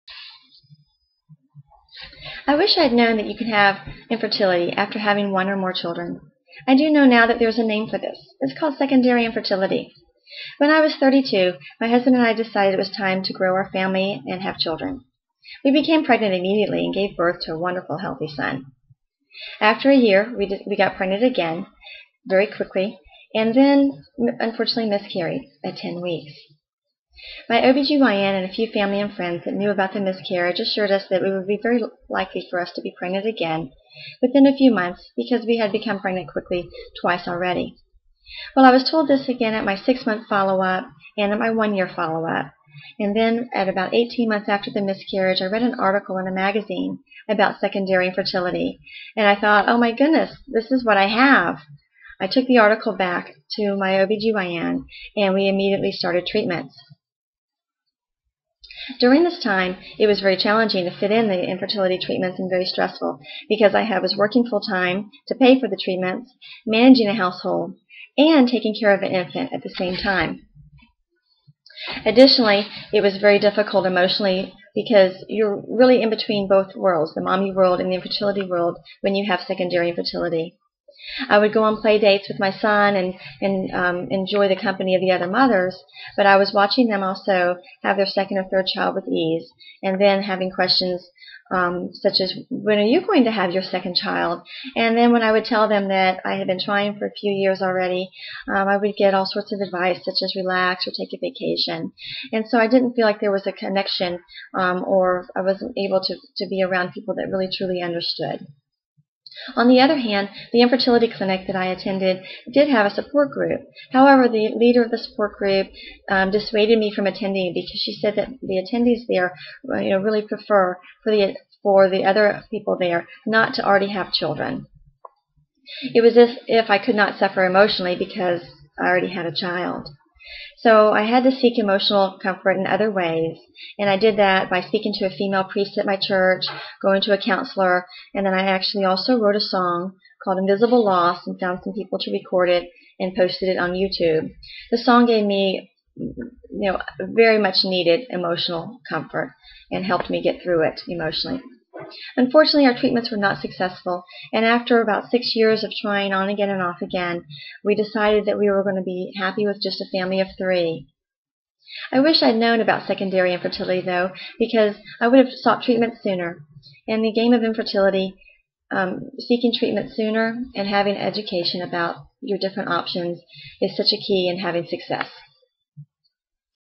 "You can experience infertility after having a child."
Deborah chronicles how challenging it was emotionally to deal with secondary infertility.